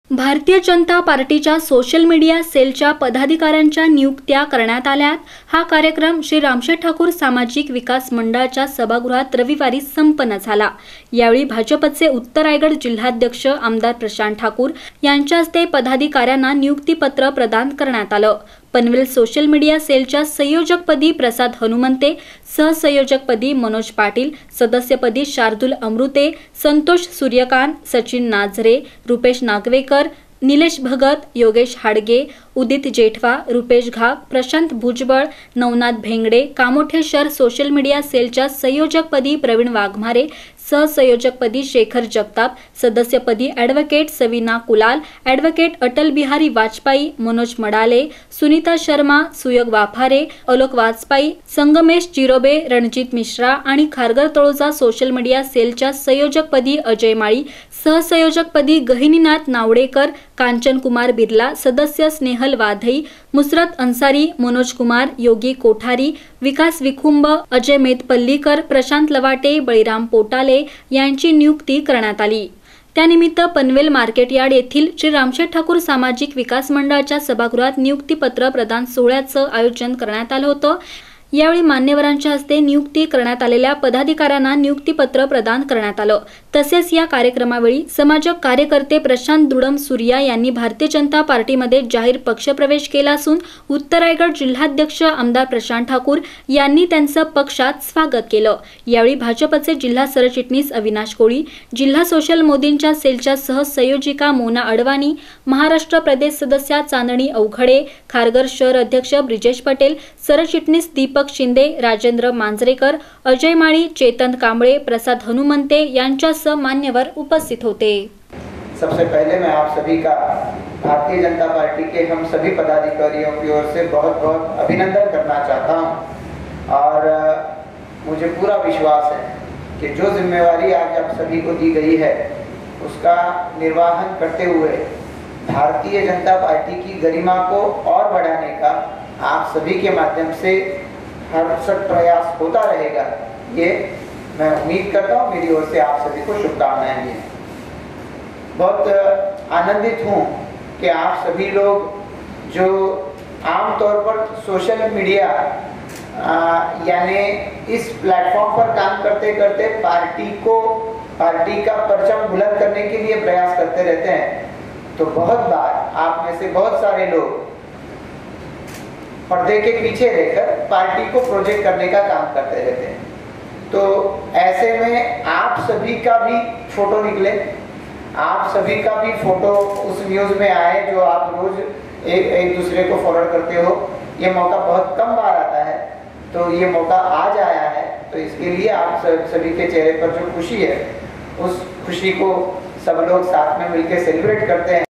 भारतीय जनता पार्टीच्या सोशल मीडिया सेलच्या पदाधिकाऱ्यांच्या करण्यात आल्या नियुक्त्या !
malhartvnews at panvel | भारतीय जनता पार्टीच्या सोशल मीडिया सेलच्या पदाधिकाऱ्यांच्या करण्यात आल्या नियुक्त्या; भाजपचे उत्तर रायगड जिल्हाध्यक्ष आमदार प्रशांत ठाकूर यांच्या हस्ते पदाधिकाऱ्यांना नियुक्ती पत्र प्रदान !